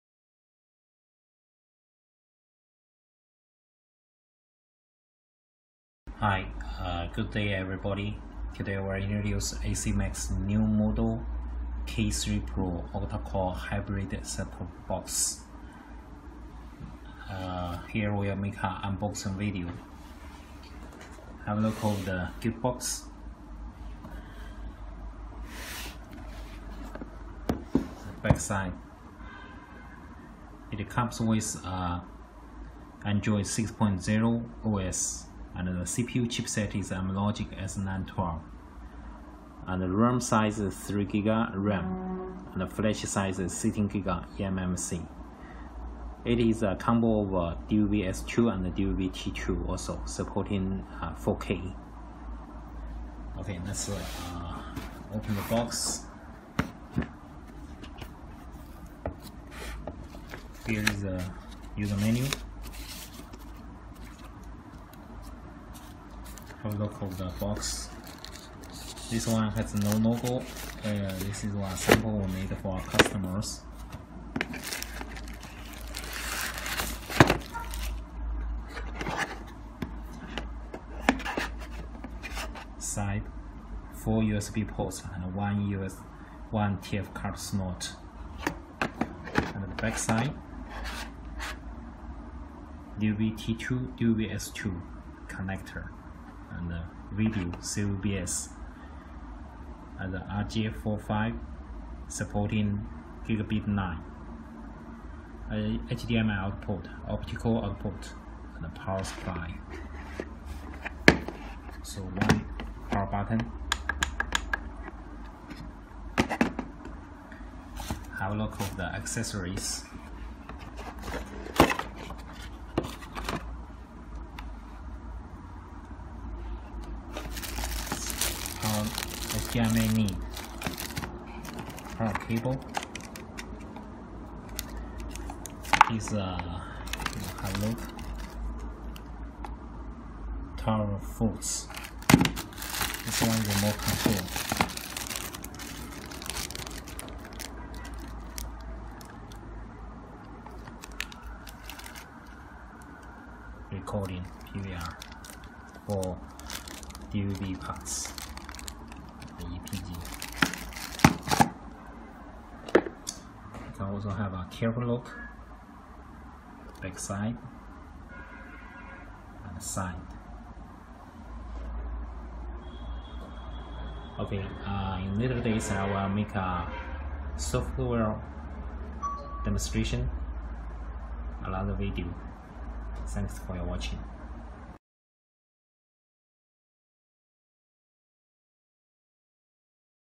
ACEMAX KIII PRO UNBOXING VIDEO
ACEMAX amlogic s912 octa core Hybrid TV BOX,  3GB RAM, 16GB ROM,dual wifi 2.4G 5.8G , 1000M internet, bluetooth, DVB-S2 DVB-T2 4K, cccam, Biss, , PowerVU supporting. 4K, Vp9 VP10, HDR, HLG